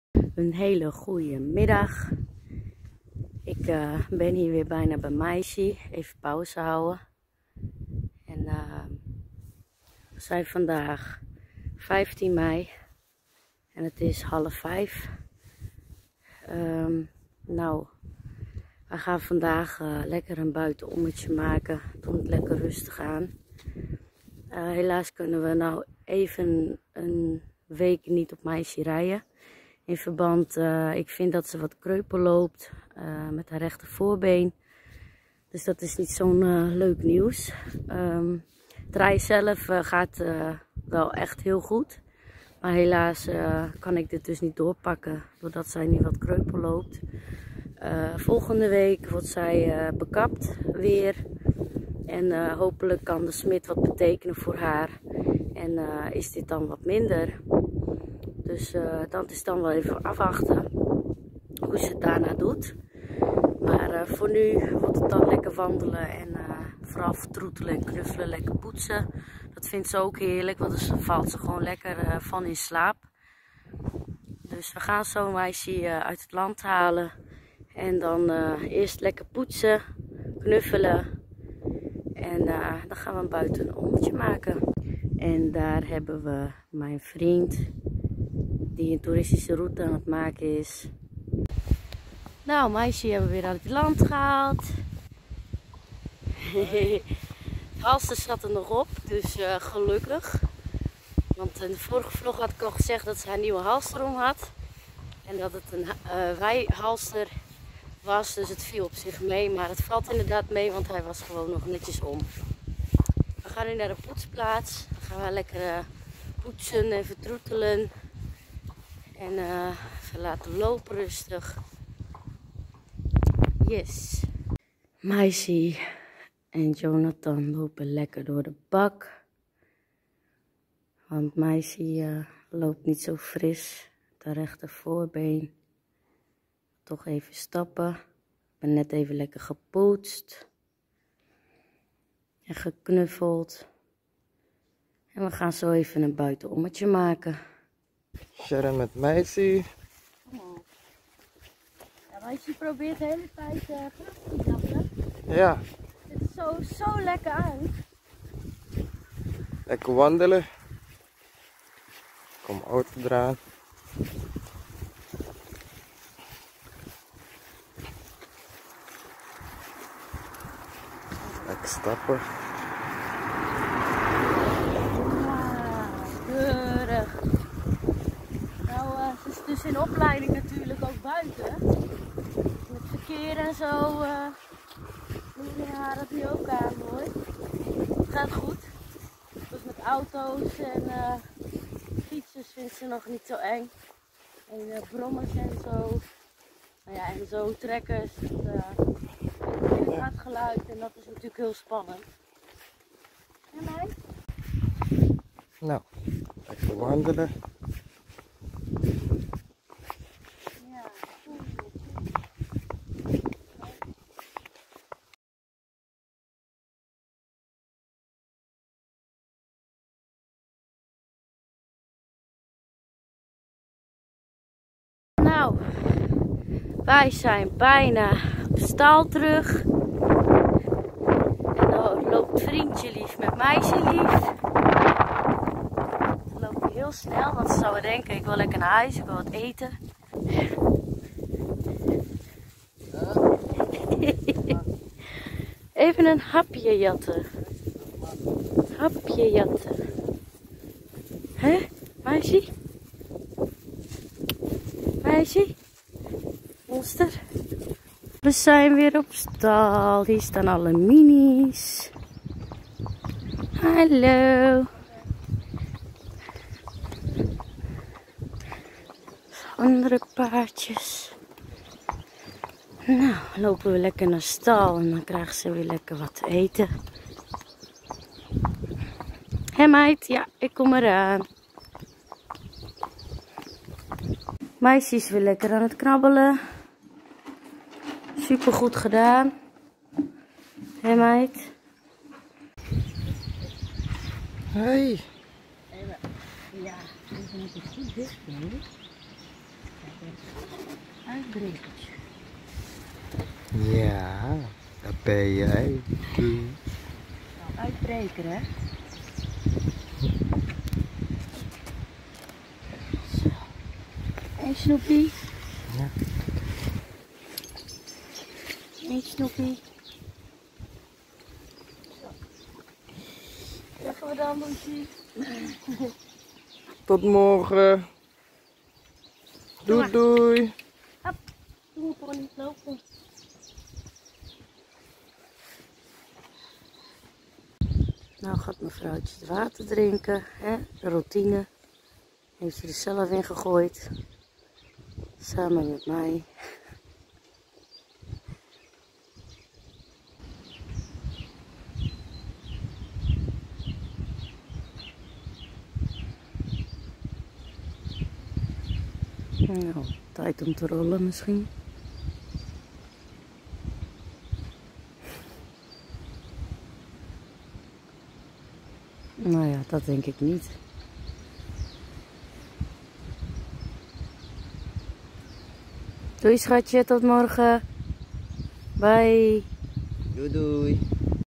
15mei verveelend nieuws!! Samen met vriend lieff bij meissie♡
Helaas verveelend nieuws voor meissie maar daar laten we ons dag zeker niet door verpesten. Dus zijn we lekker buiten om gaan wandelen. Hopelijk kan de hoefsmid ons helpen en kunnen we het doorrijden weer opakken.